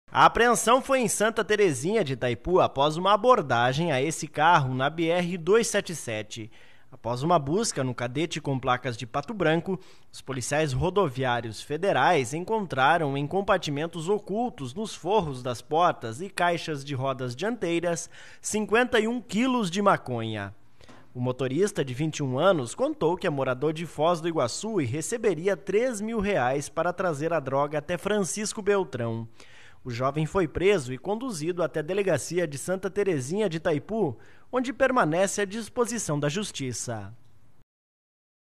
Polícia apreende drogas que seriam trazidas para Francisco Beltrão
Os policiais encontraram 51kg de maconha escondidas em um carro
abordado em Santa Terezinha de Itaipu.